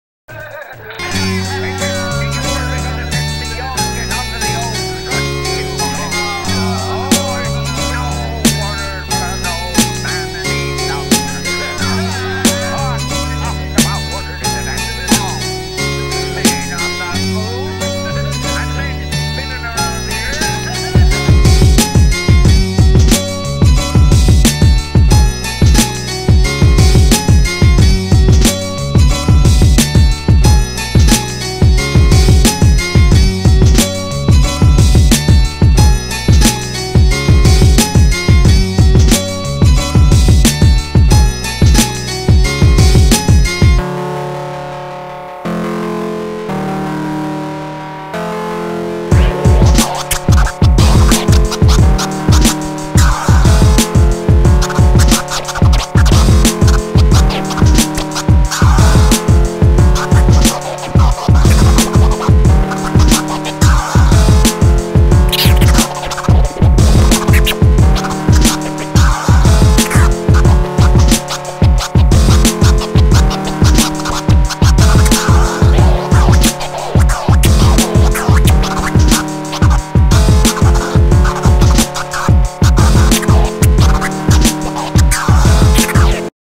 SDAM_promo.mov
San Diego Action Media's "New Logo" Promo video. Shot by Ian Robertson & Dan Coronado on a Canon 7d. Music by DJ Kechup. Edited by Ian Robertson. Please Visit sdactionmedia.com for more info!